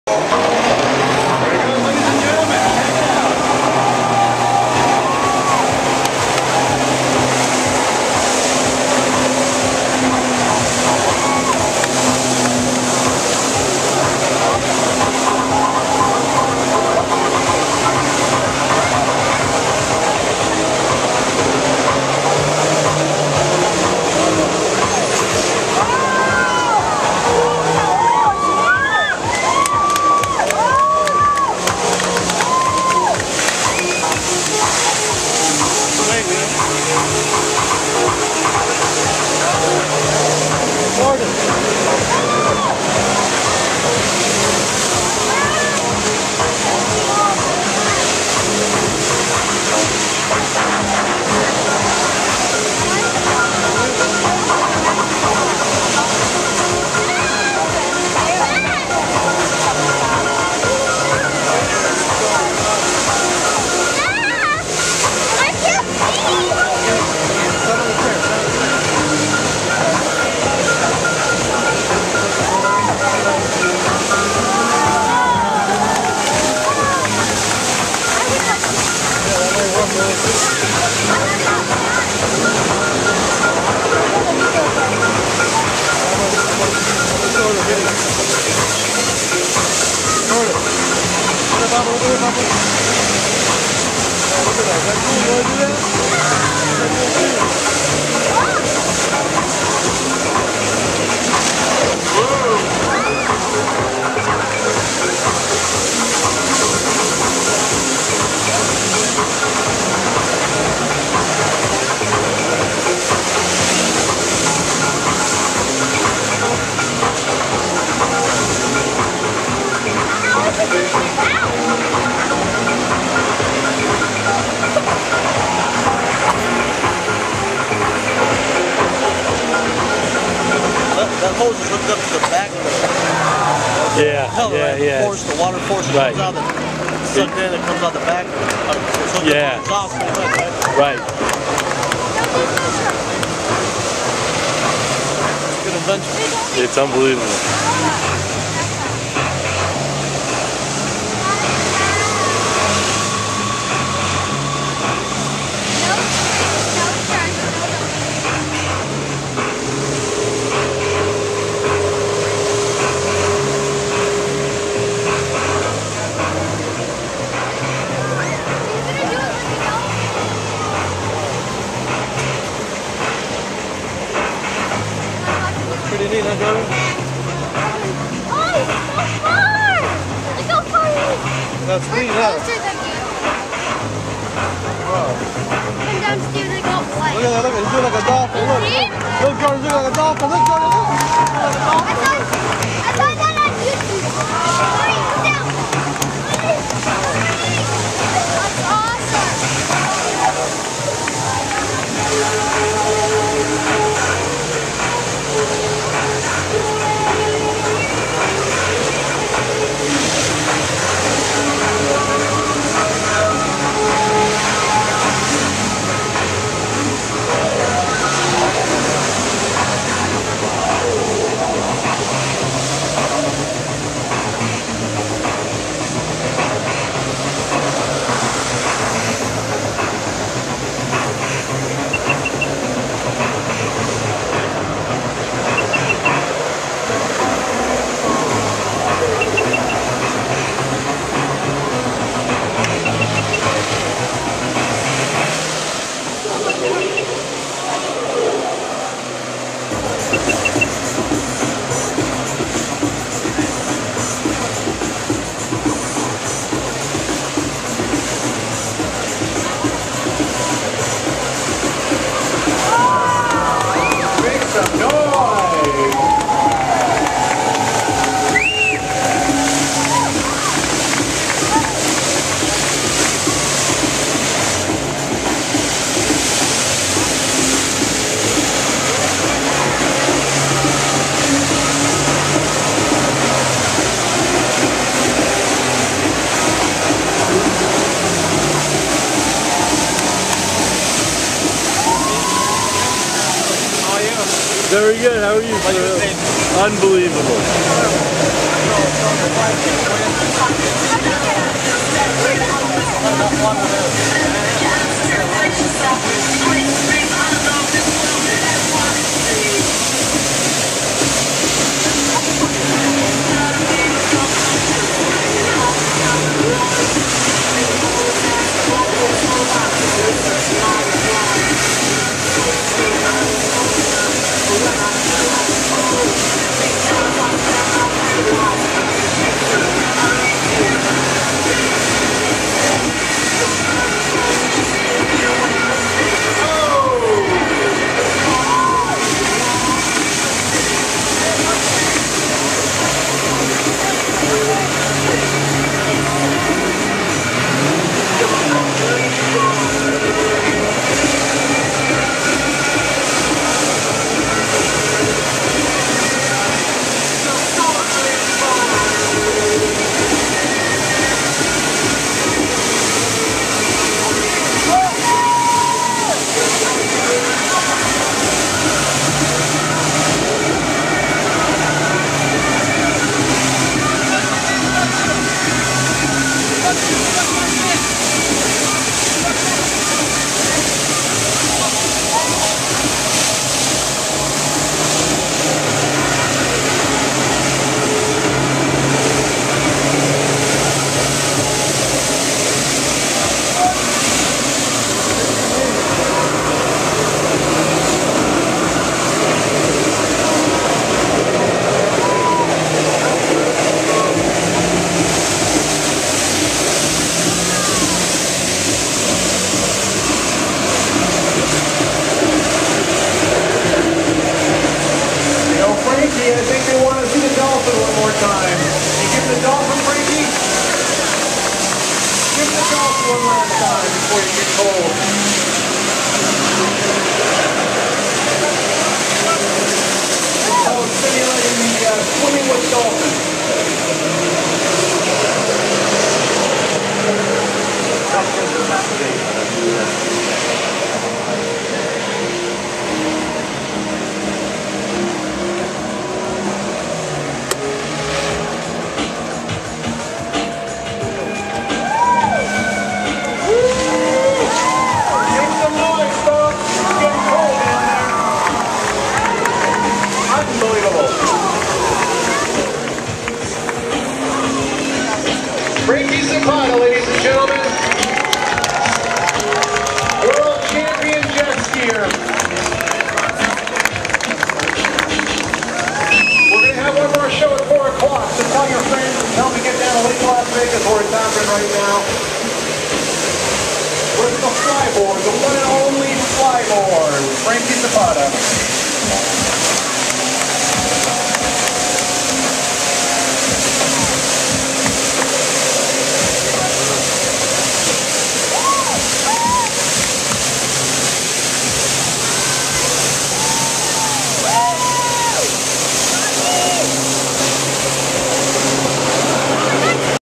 FLYBOARD ZAPATA
Franky Zapata debuts his one of a kind, 1st ever in the USA, the fly board! From France, the world champion jet ski racer Franky Zapata invented, created brought to market the fly board, which is an attachment that hooks up to your Jet Ski and allows you to fly! Fox 5 news, CNN and Channel 8 news were all there to capture this event at Lake Las Vegas. If you're interested in this product for your event, please